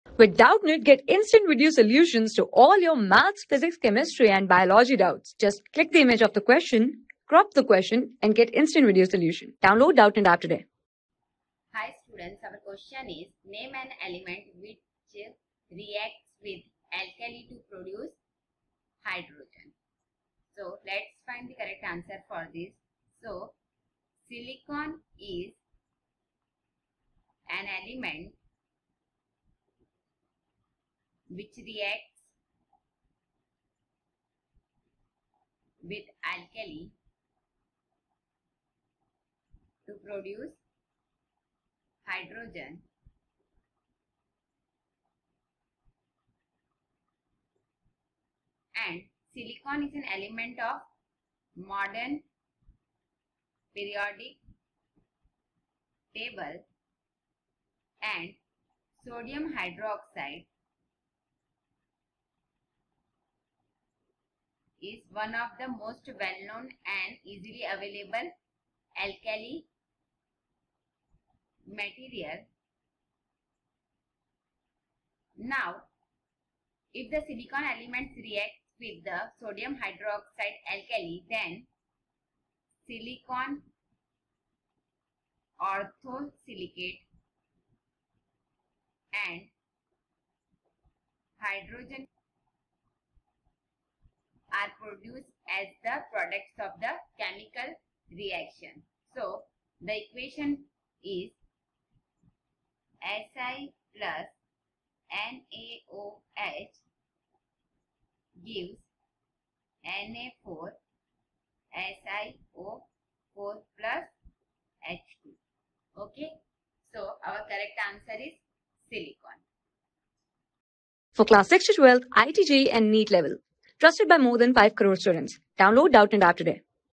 Name An element which reacts with alkali to produce hydrogen. | 9 | SPECIMEN PAPER 3 | CHEMISTRY...
Name An element which reacts with alkali to produce hydrogen.
Class: 9
Subject: CHEMISTRY
Chapter: SPECIMEN PAPER 3
Board:ICSE

👉 Have Any Query? Ask Us.